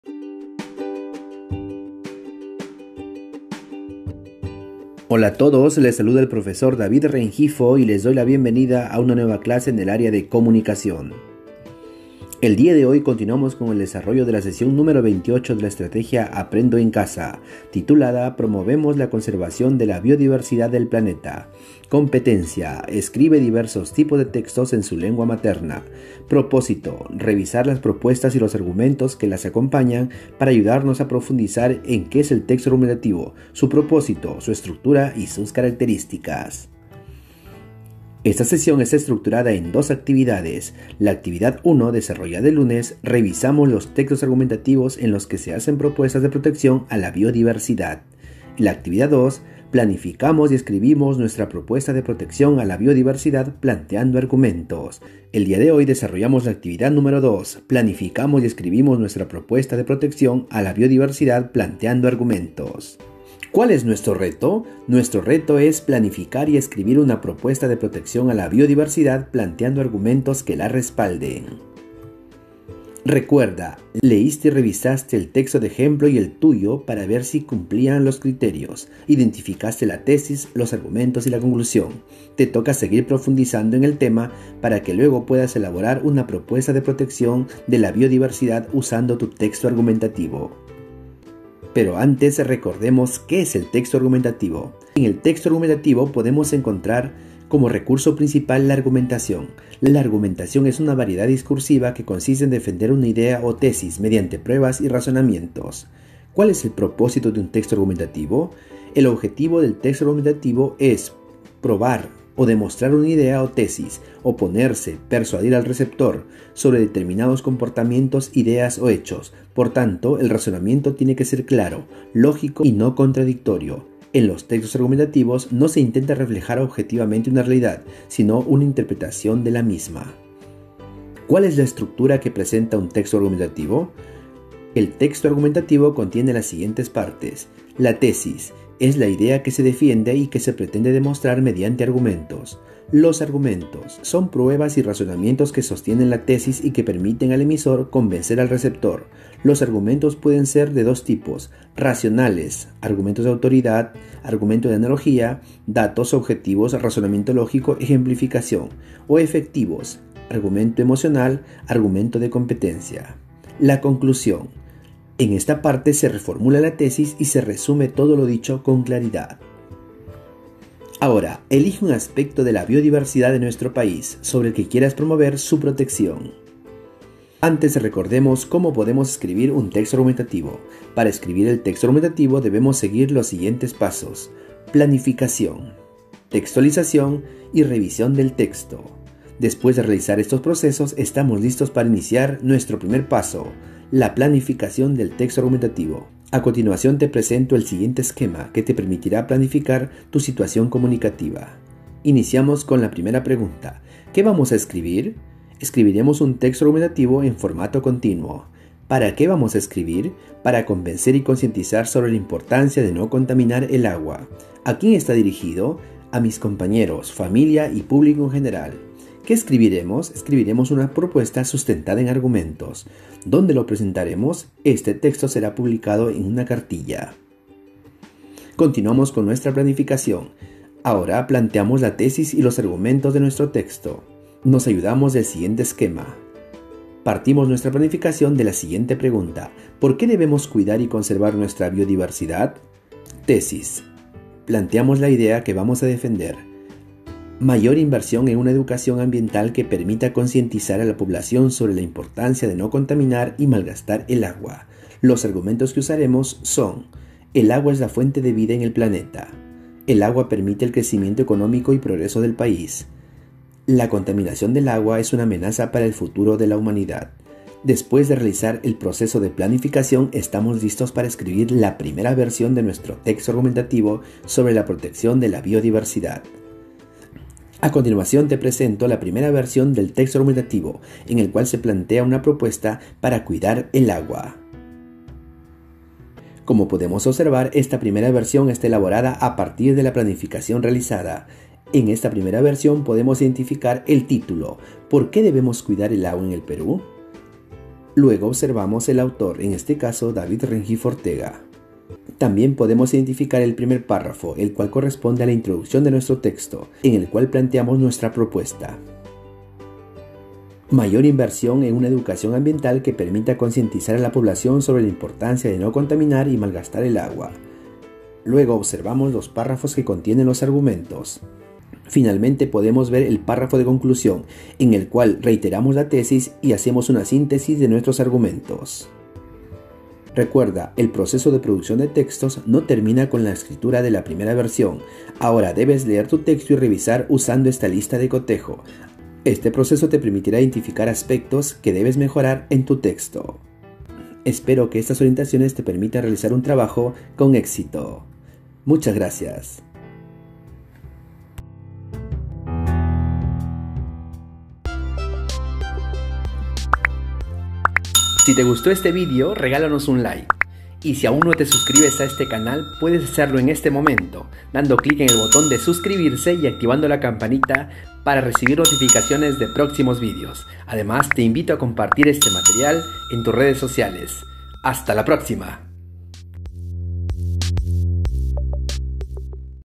🟨PLANIFICA Y ESCRIBE UN TEXTO ARGUMENTATIVO | APRENDO EN CASA COMUNICACIÓN SEMANA 28 (DÍA 2)
PLANIFICA Y ESCRIBE UN TEXTO ARGUMENTATIVO SOBRE LA IMPORTANCIA DE PROTEGER LA BIODIVERSIDAD | APRENDO EN CASA COMUNICACIÓN SEMANA 28 (DÍA 2)